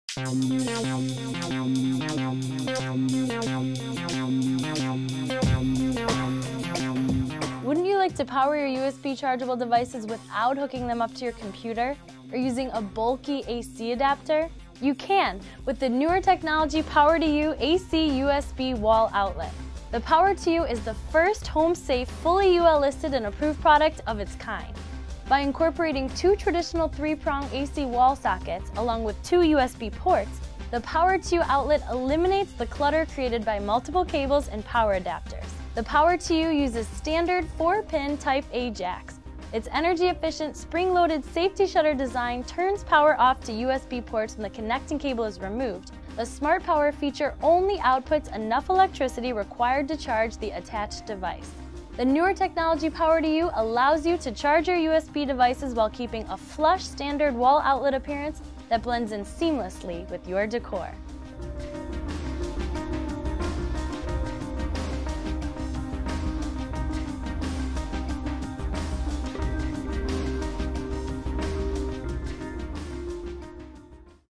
A Closer Look - Power2U AC/USB Wall Outlet by Newer Technology
The Newer Technology Power2U AC/USB Wall Outlet is the safe in-wall solution for conveniently powering and charging USB devices at home. Now you can charge and/or power up to four devices at once when using the USB 3.0/2.0 ports and standard three-prong outlets, while eliminating unsightly clutter created by multiple cables and bulky power adapters.

Ideal Home Upgrade For Multiple Device Users
Ideal for homes with users of multiple USB chargeable devices, the Power2U AC/USB Wall Outlet is a basic "DIY" install on any 15Amp circuit with a 16 cubic inch electrical box** to blend seamlessly into the wall as a standard power outlet. A "straightforward" rated "how to" video demonstrates how the Power2U can be installed in as little as 20 minutes.

Money Saving, Energy Efficient Design
Offering a modern, centralized location for charging/powering USB devices, the Power2U offers two energy efficient features to complement its use convenience. The Smart Power design only outputs enough charging power required by the attached device, while spring-loaded Safety Shutters turn power off to the USB ports when a connecting cable is removed.

Install With Confidence
Power2U AC/USB Wall Outlet is the first home-safe, fully UL Listed and approved product of its kind! Beware of knockoffs and other similar in-wall solutions which misrepresent or do not have verified UL approval.